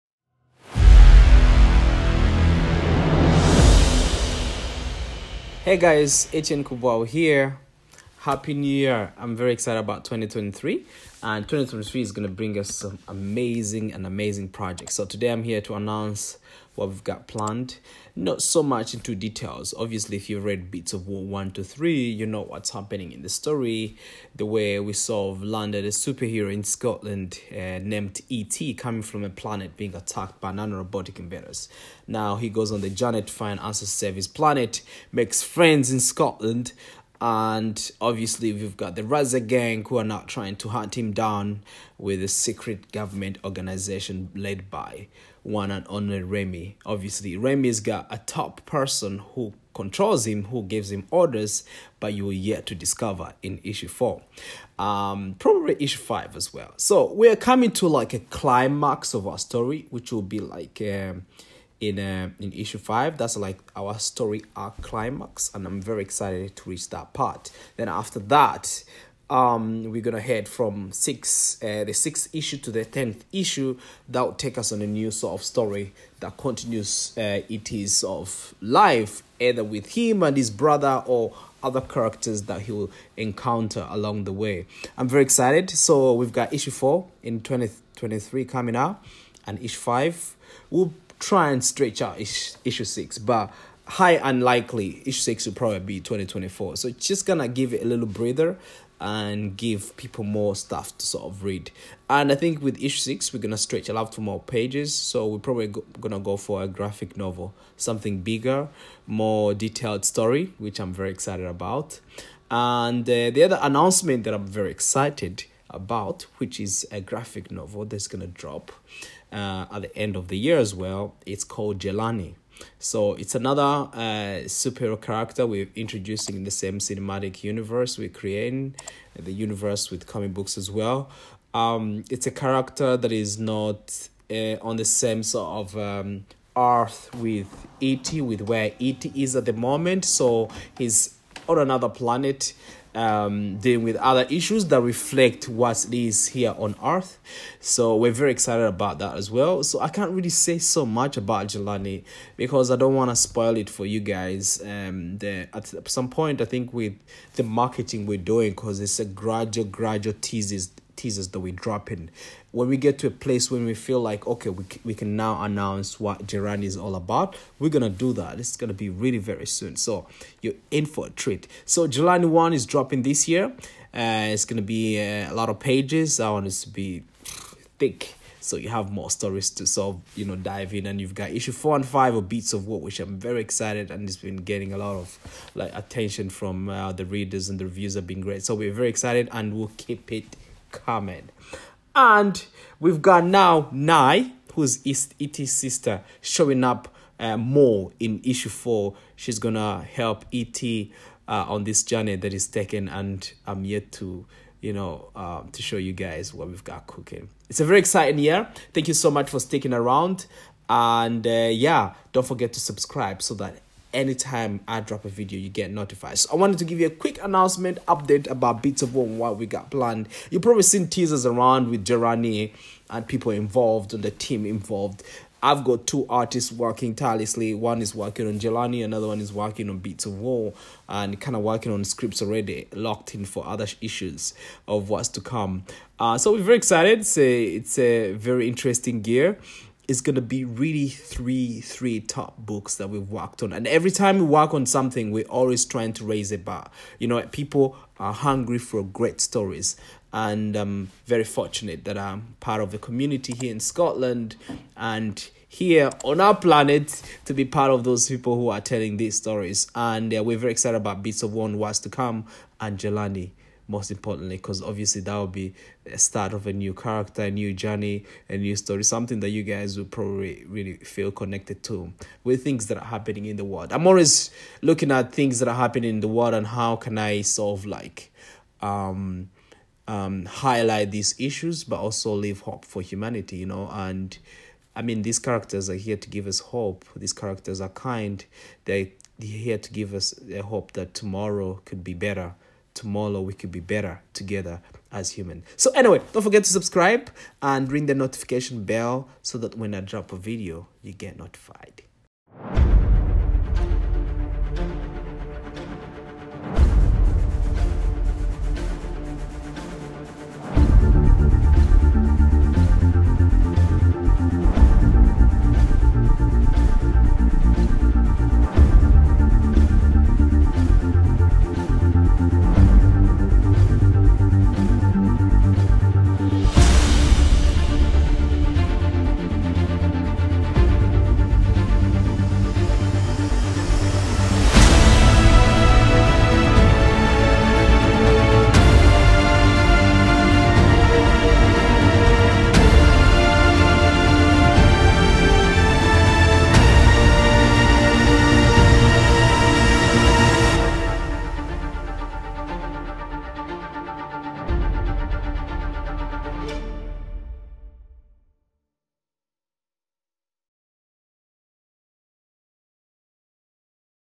BEATS OF WAR AND JELANI UPDATE | THE ETIAN UNIVERSE 2023
Welcome to KƎ COMICS'  Etian Universe

Support our comic books by purchasing directly from our website.
Purchase BEATS OF WAR  1
Synopsis:
An alien planet ravaged by robot invaders
A city on earth plagued by a ruthless gang
Their only hope: a hero from beyond the stars

Synopsis:
An alien visitor continues his quest to save his planet. A ruthless gang seeks revenge for an untimely death. Meanwhile, government agents are on the hunt... And the beats of war drum louder.

Synopsis:
An alien visitor gets one step closer to achieving his mission. His sister brings back memories of a difficult past. Meanwhile, government agents are on the attack... And the beats of war drum even louder

► Discover Beats of War on Amazon/Kindle: